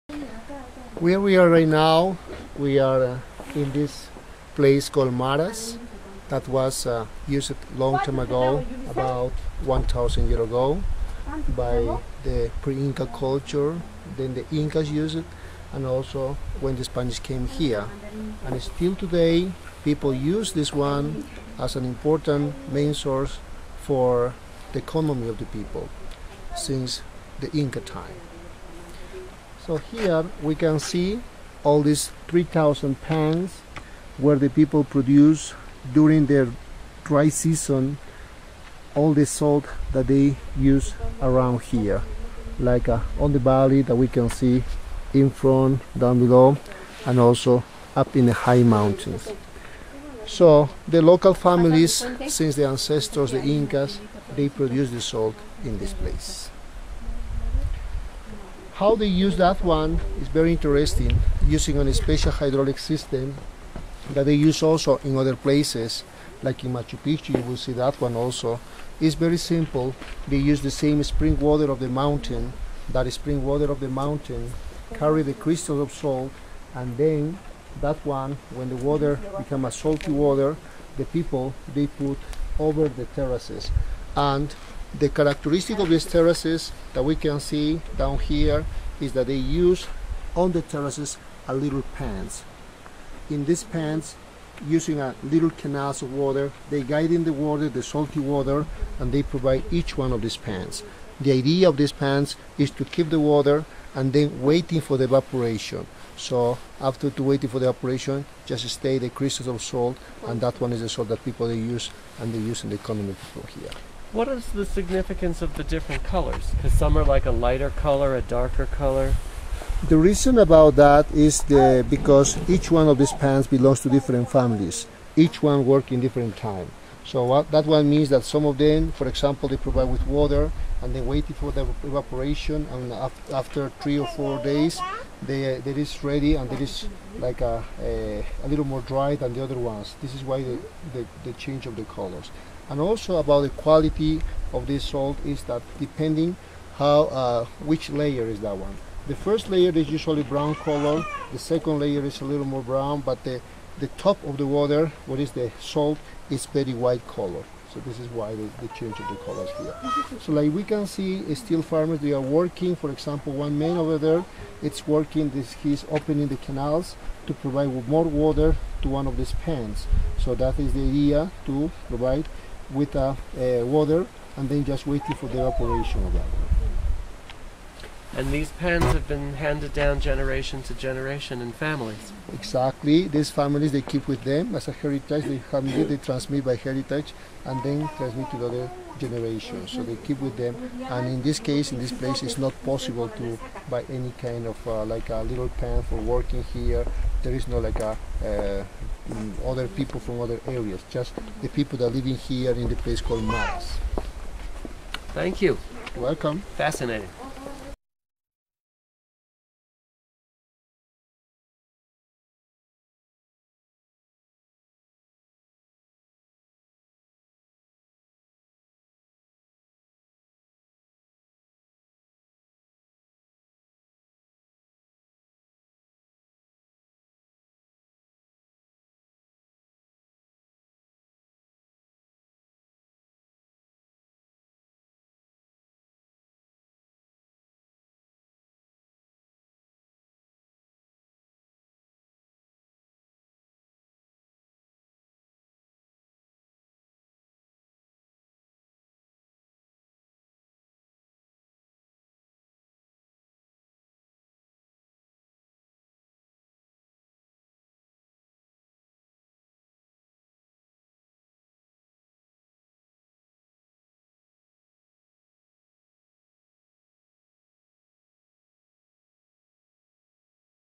Maras Salt Pans Peru 2011
Gary Stevens Visits the Maras Salt Pans in Peru.  Visit Gary's blog: garythetravelexpert.blogspot.com